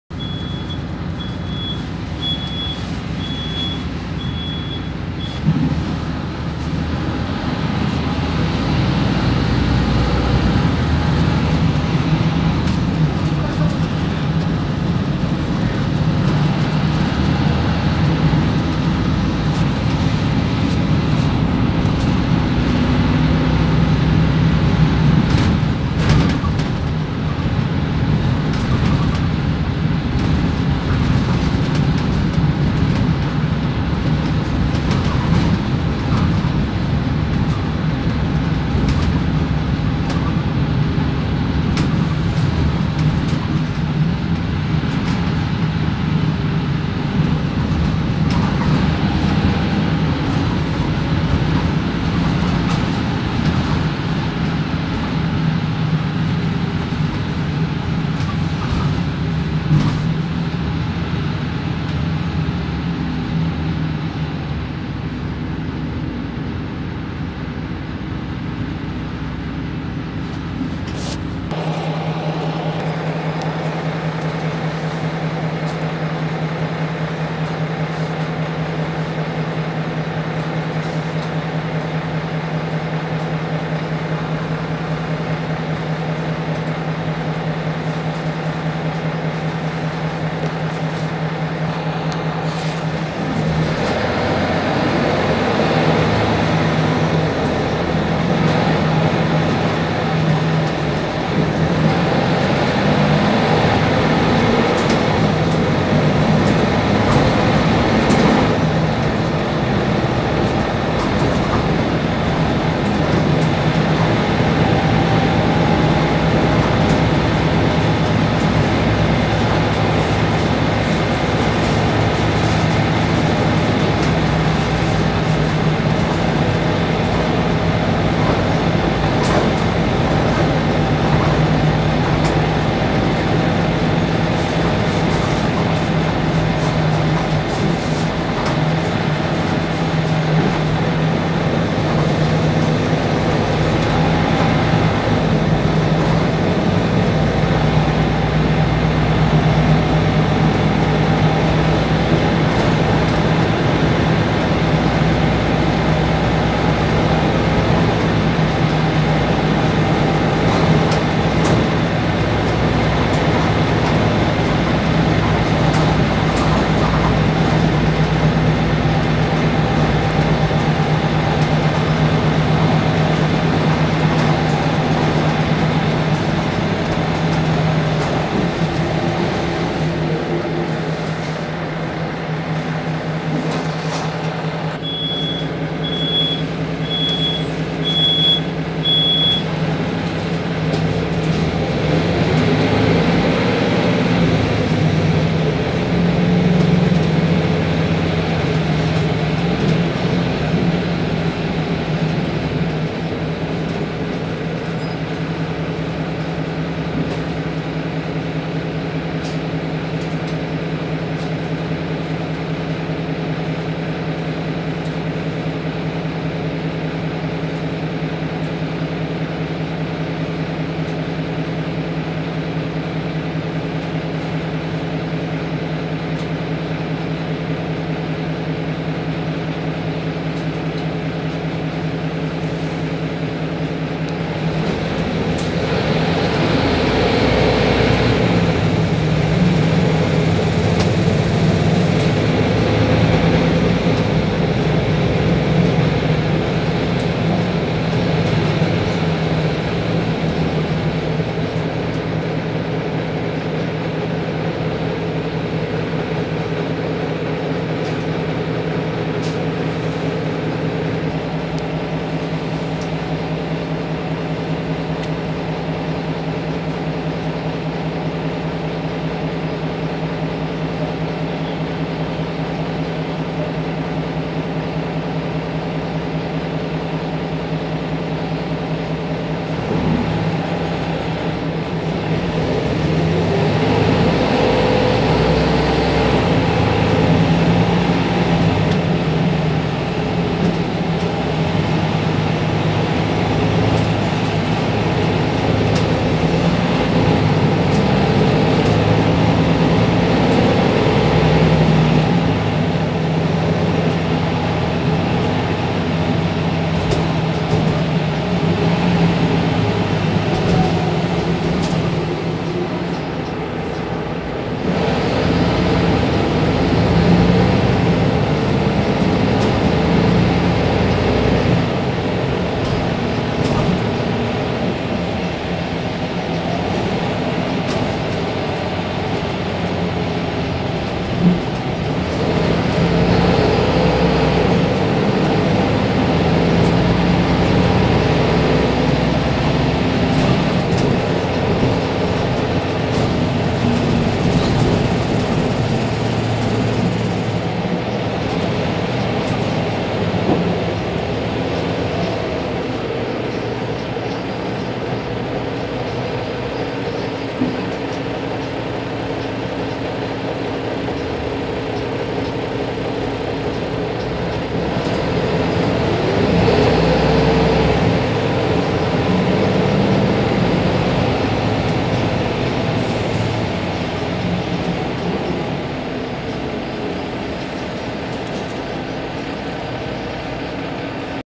RWA-542 [KeKo Kft.] ITK Mercedes-Benz Reform 500LE (audio/hangfelvétel)
Rendszám: RWA-542
Típus: ITK Mercedes-Benz Reform 500LE
Üzemeltető: KeKo Kft.
Járat amin készült: Kecskemét, 1-es járat
Selejtezve: Nincs
Gyártási év: 2020
Motor: OM936 Euro6
Váltó: ZF-EcoLife
Felvétel készült: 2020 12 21
Ajánlatos rész: 1:32 , 3:08